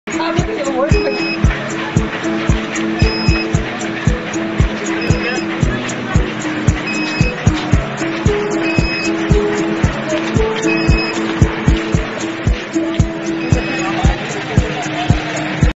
K3 Upgraded Infrared Thermometer---Comparison of infrared temperature measurement speed
K3 Upgraded Infrared Thermometer---Comparison of infrared temperature measurement and ear thermometer temperature measurement speed